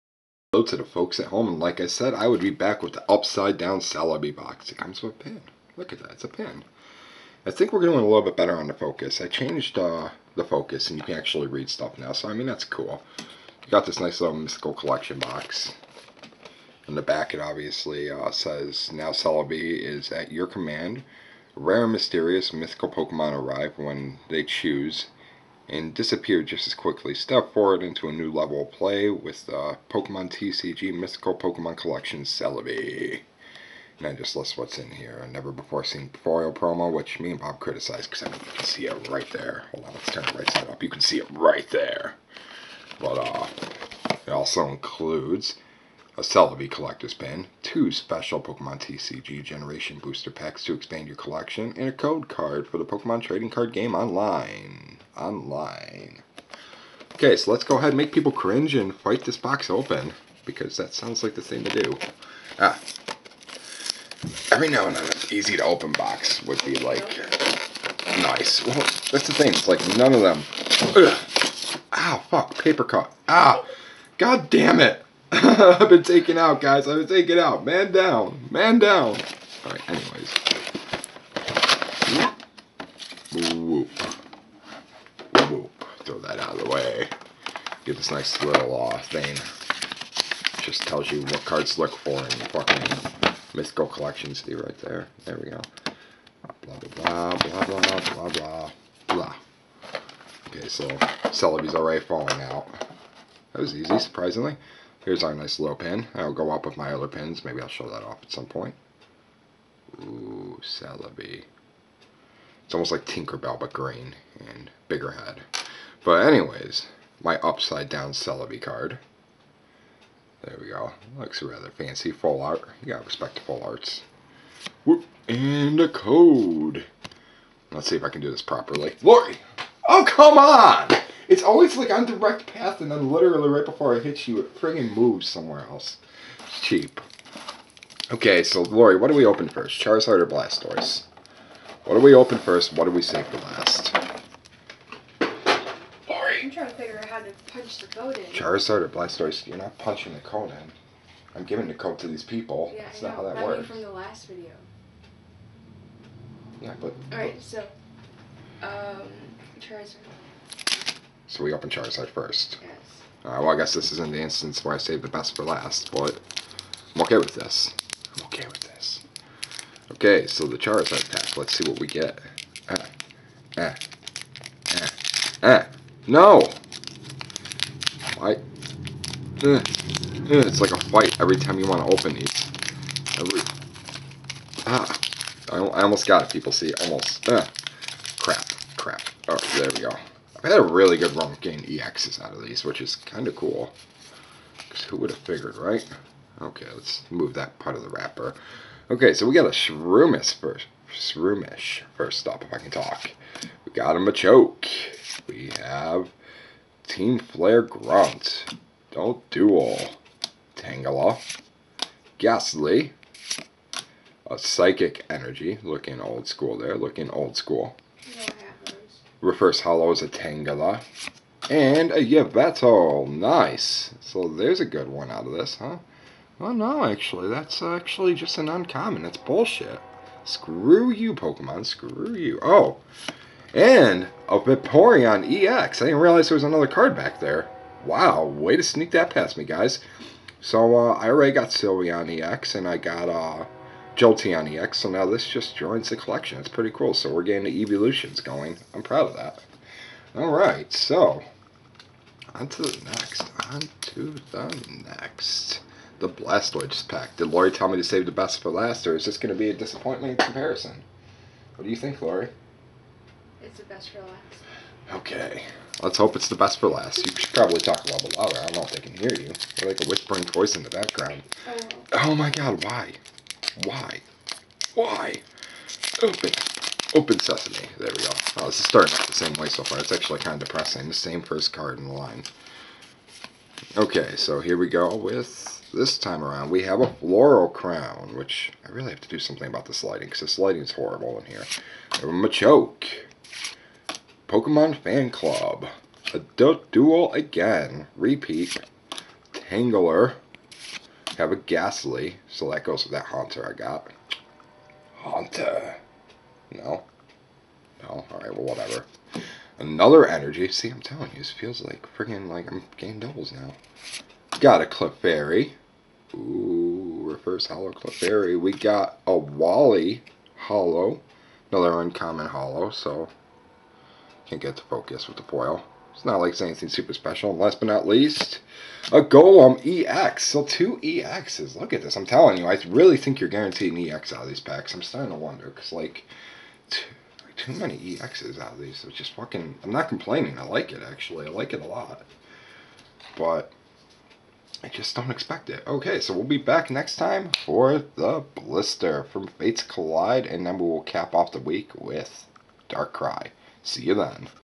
Pokemon TCG - Mythical Collection Celebi Box
The opening of Pokeman Cards, cause it's cool!